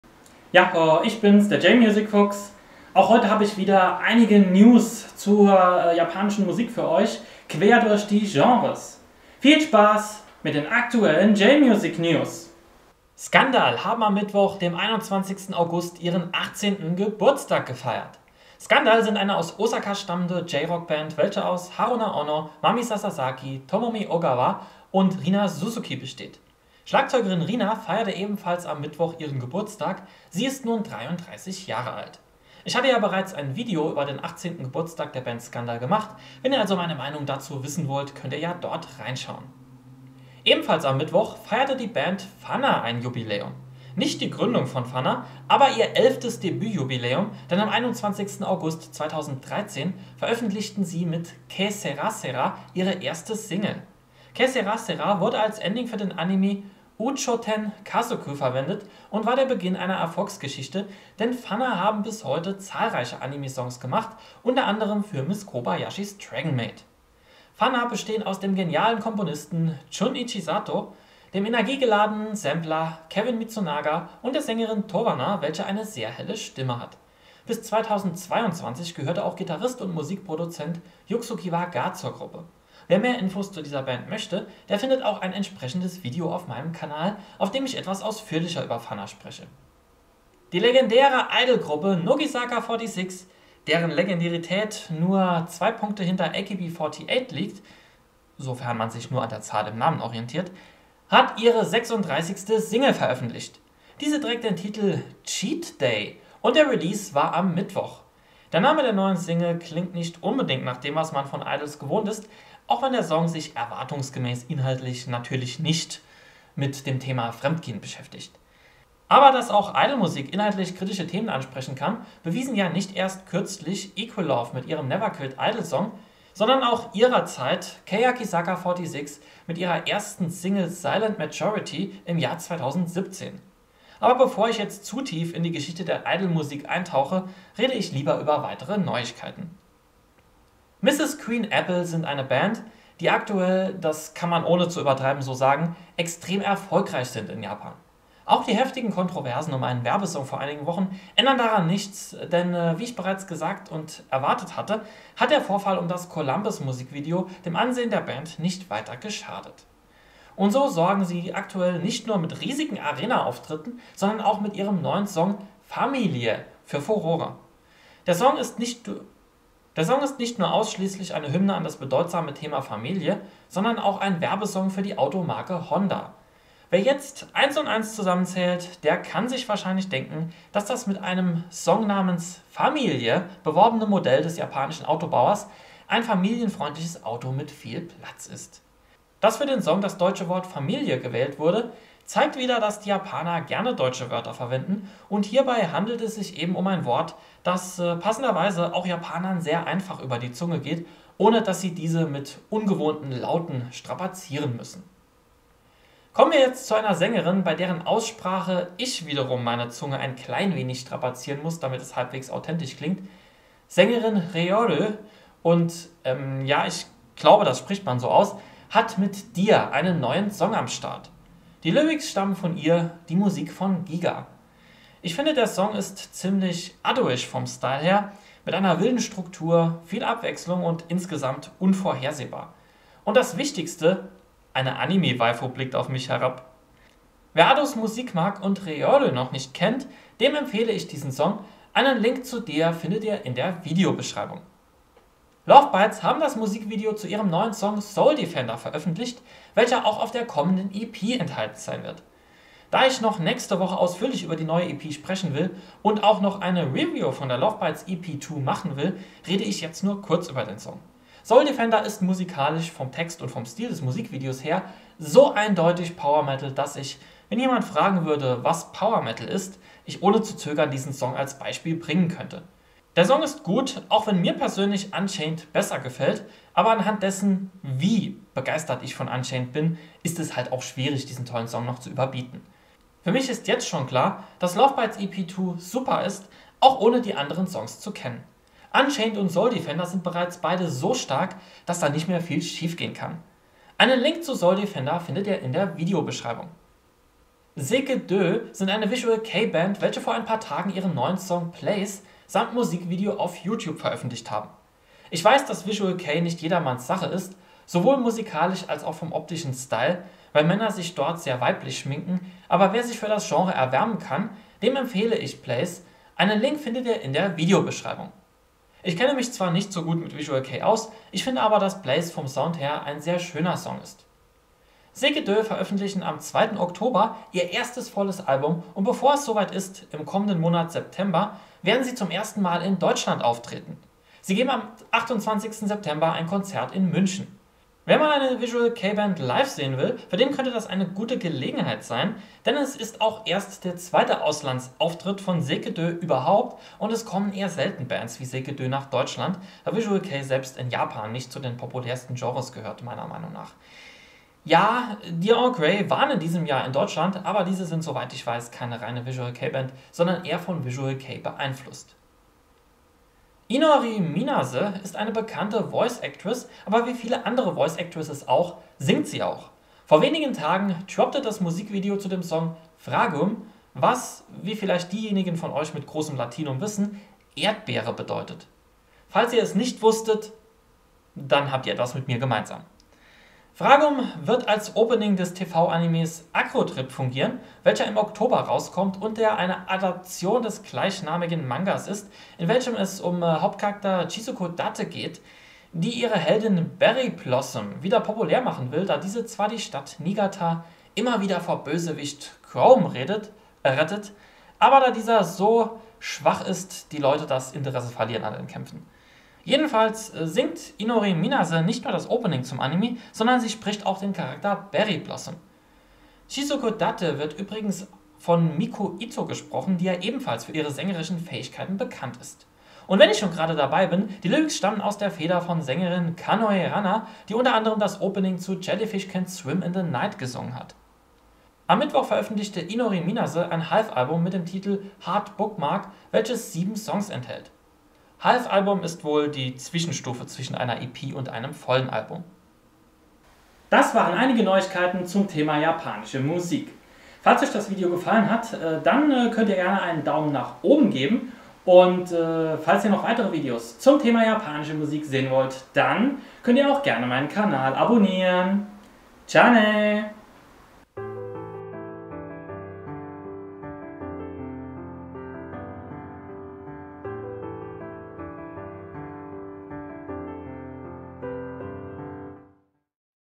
Aktuelle J-Music-News!!! #40
In diesem Video rede ich über aktuelle Neuigkeiten zum Thema Japanische Musik! Dabei geht es unter anderem um fhána, Mrs. GREEN APPLE und LOVEBITES!

Inhalt:

00:00 Einleitung
00:13 SCANDAL
00:40 fhána
01:30 Nogizaka46
02:20 Mrs. GREEN APPLE
03:25 Reol
04:05 LOVEBITES
04:57 Zeke Deux
06:03 Inori Minase
07:23 Schlussteil

3 von oben links beginnend und den Einzelbildern im Uhrzeigersinn folgend:
Priya Rathore, CC0, via Wikimedia Commons

Bearbeitung Bilder: Text eingefügt, Thumbnail bearbeitet

Ich garantiere nicht für die inhaltliche Richtigkeit der Angaben in diesem Video.